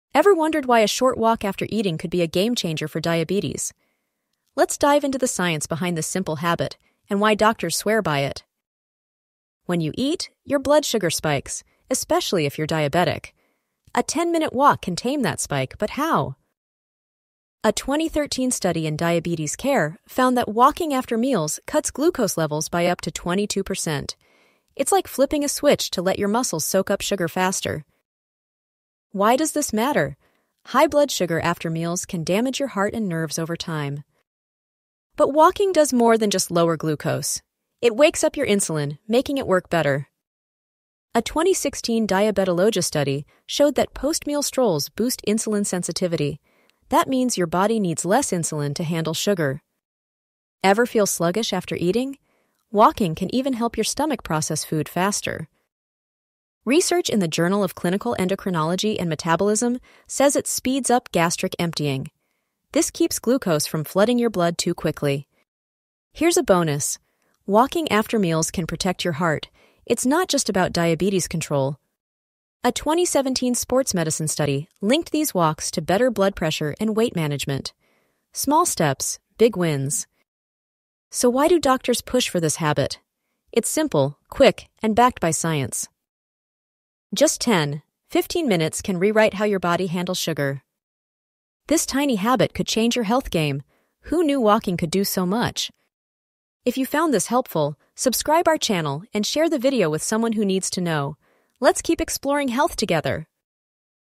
Science Backed Trick to Reduce Glucose Spike Without Medicine !
Discover why walking for 10-15 minutes after meals is a powerful tool for managing diabetes! Backed by science, this video explains how a simple stroll can lower blood sugar, boost insulin sensitivity, and protect your heart. Check out the studies below for a deeper dive into the research:

DiPietro, L., et al. (2013). "Three 15-min bouts of moderate postmeal walking significantly improves 24-h glycemic control in older people at risk for impaired glucose tolerance." Diabetes Care.

Reynolds, A. N., et al. (2016). "Advice to walk after meals is more effective for lowering postprandial glycaemia in type 2 diabetes mellitus than advice that does not specify timing." Diabetologia.

Frank, P., et al. (2018). "Effects of exercise on gastric emptying and glucose metabolism in humans." The Journal of Clinical Endocrinology & Metabolism.

Boule, N. G., et al. (2017). "Effects of exercise on glycemic control and body mass in type 2 diabetes mellitus." Sports Medicine.